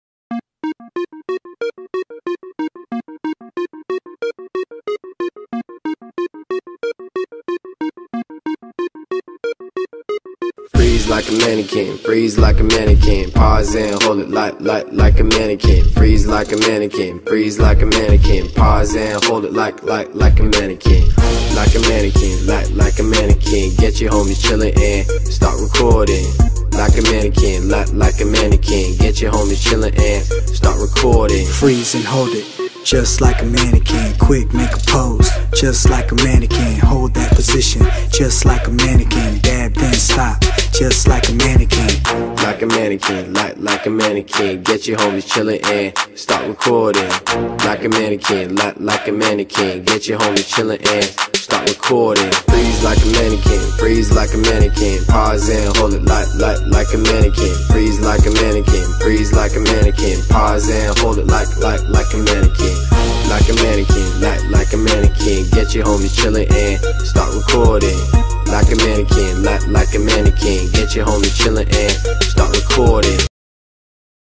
Il mannequin challenge dei giovani Anpas
26 novembre, Genova: il mannequin challenge organizzato dai giovani Anpas all'11^Conferenza di Organizzazione Anpas